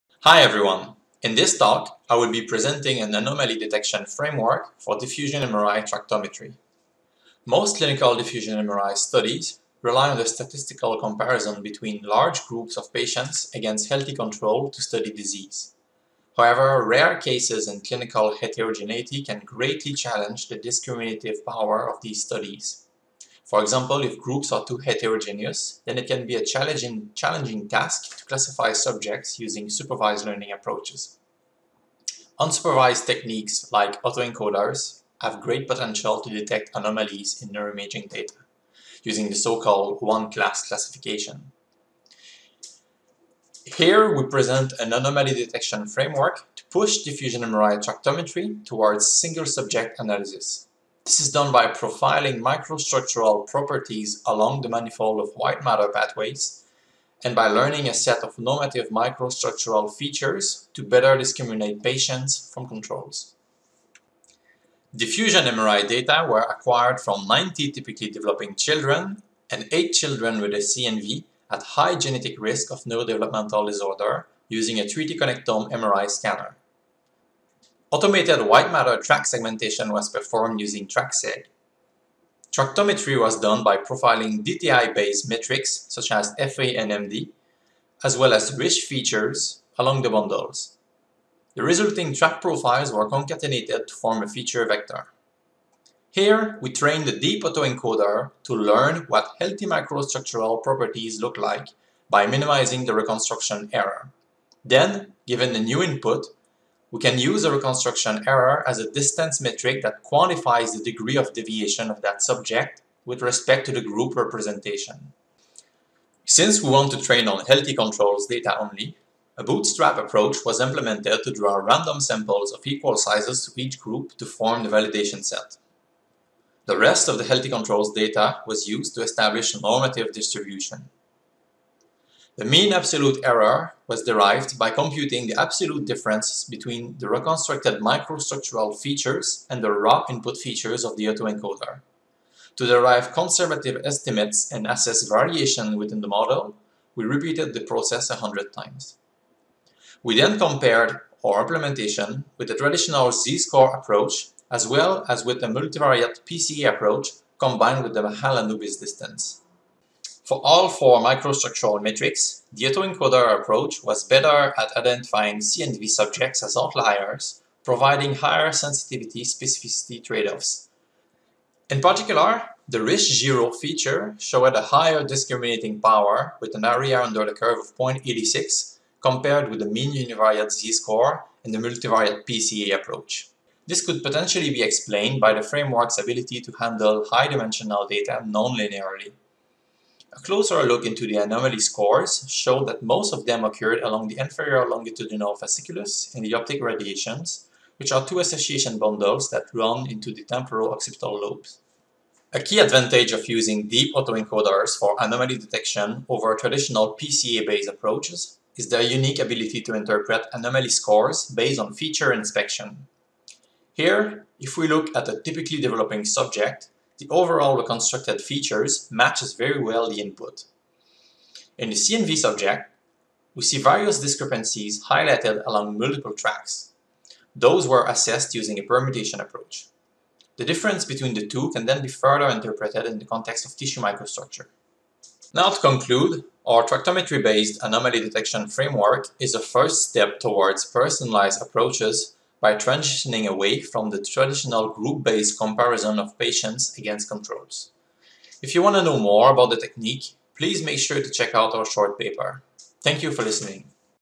MIDL 2020, S027, Chamberland et al. Short presentation
S027 - Tractometry-based Anomaly Detection for Single-subject White Matter Analysis

Maxime Chamberland, Sila Genc, Erika P. Raven, Greg D. Parker, Adam Cunningham, Joanne Doherty, Marianne van den Bree, Chantal M. W. Tax, Derek K. Jones

Abstract: There is an urgent need for a paradigm shift from group-wise comparisons to individual diagnosis in diffusion MRI (dMRI) to enable the analysis of rare cases and clinically-heterogeneous groups. Deep autoencoders have shown great potential to detect anomalies in neuroimaging data. We present a framework that operates on the manifold of white matter (WM) pathways to learn normative microstructural features, and discriminate those at genetic risk from controls in a paediatric population.